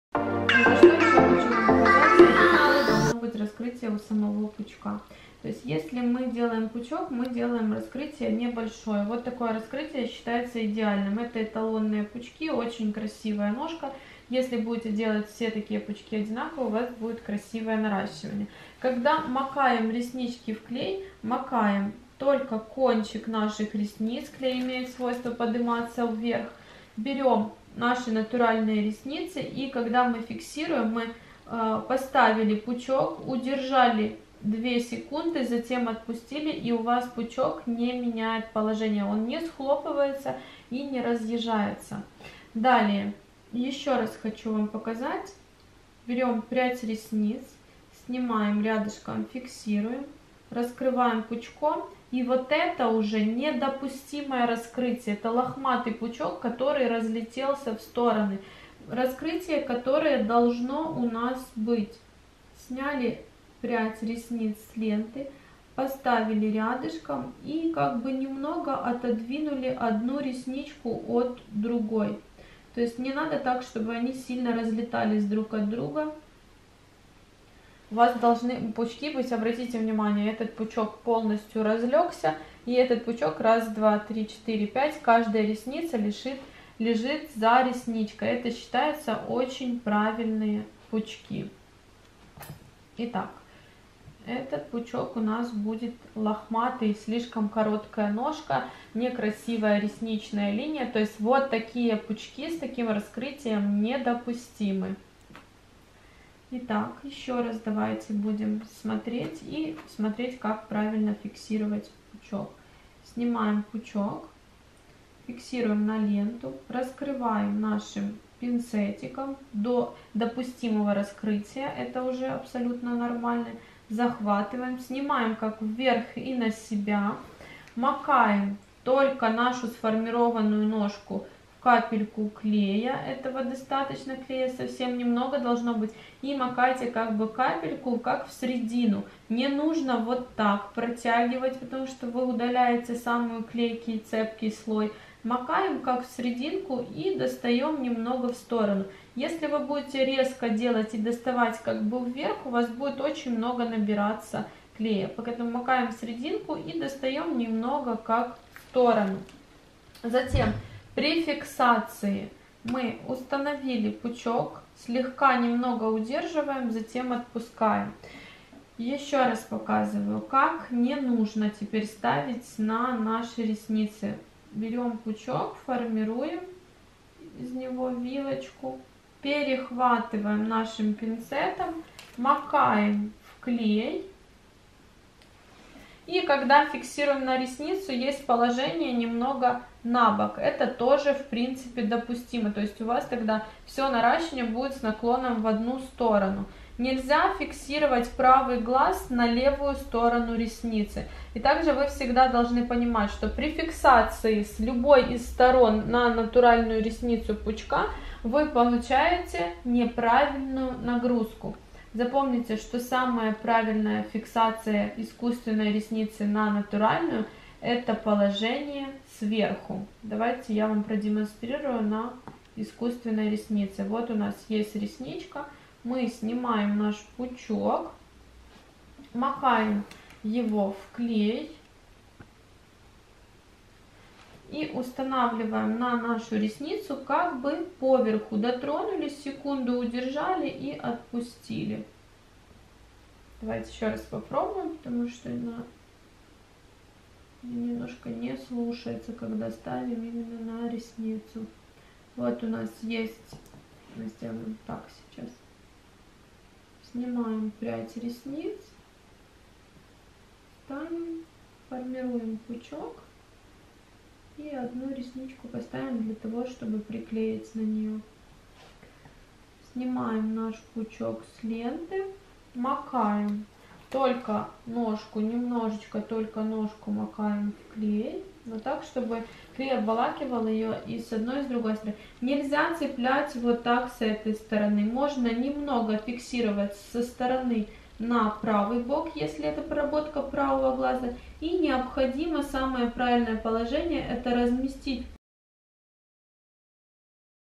Идеальный пучок существует (рассмотрим механическую ленточную технику)
В этом видео я хочу Вам показать раскрытость идеального пучка. Что бы сделать наращивание красивым необходимо соблюдать геометрию и постановку пучка на ресницу. В данном видео техника механическая или второе название ленточная) . Так же есть еще ручная техника (мне ближе) . Надеюсь мое видео Идеальный пучок существует (рассмотрим механическую ленточную технику) будет Вам интересно и полезно.